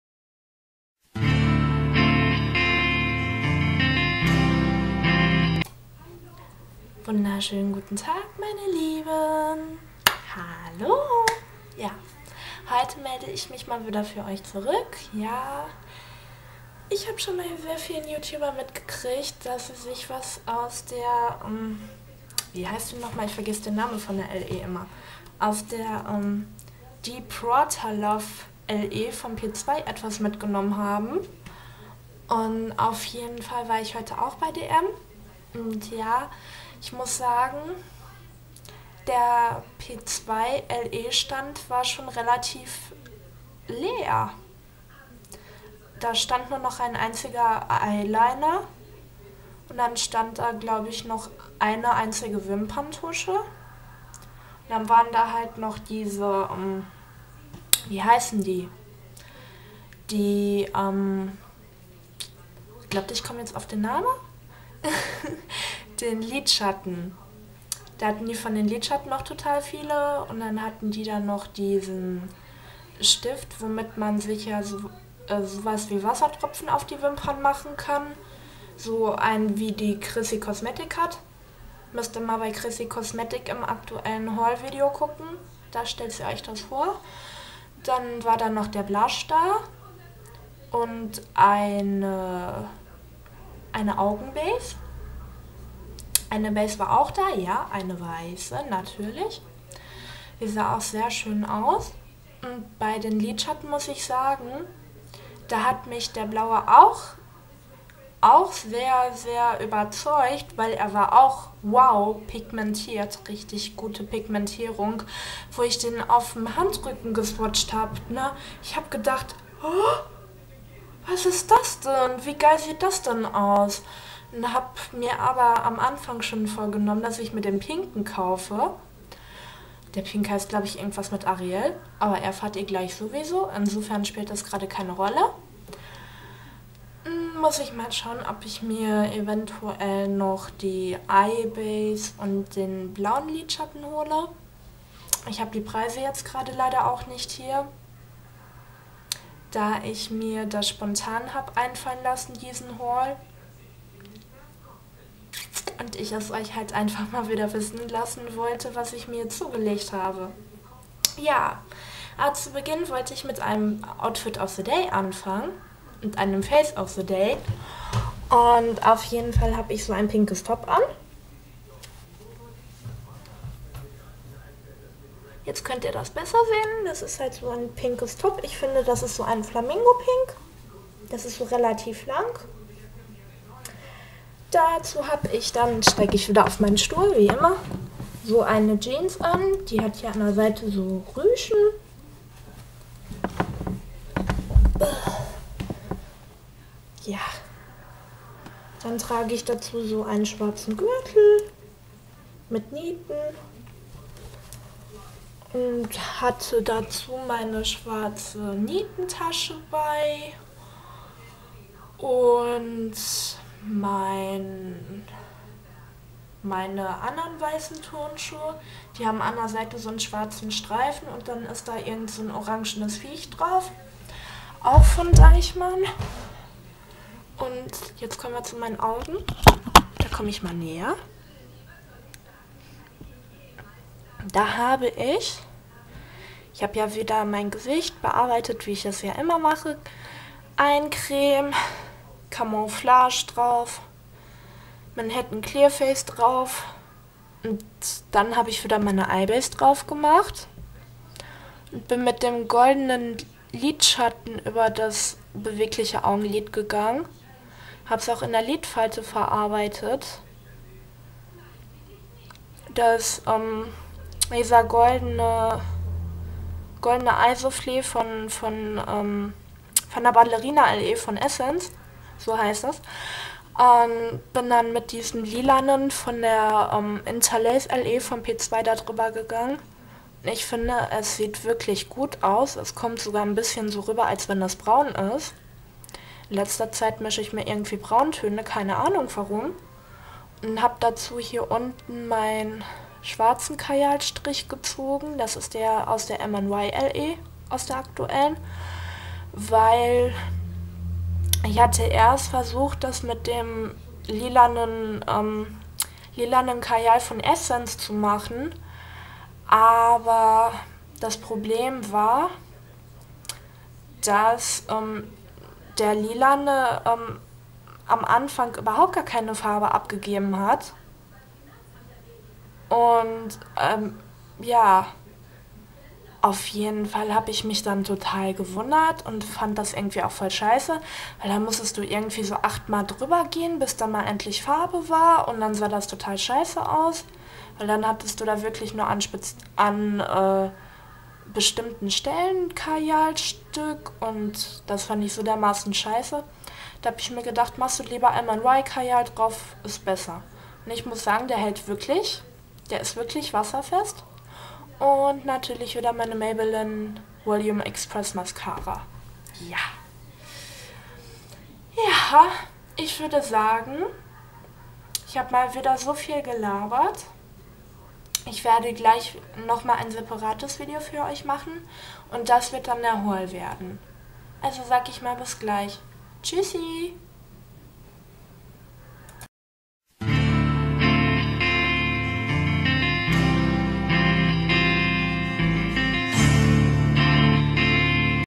ootd und fotd
das top is von madonna gabs als da sale war für 2 oder 5€
den lidschatten aus der ballerina le den goldenen und den lilanen aus der inter lace le hab ich verwendet und sie über einander gegeben und vermischt =)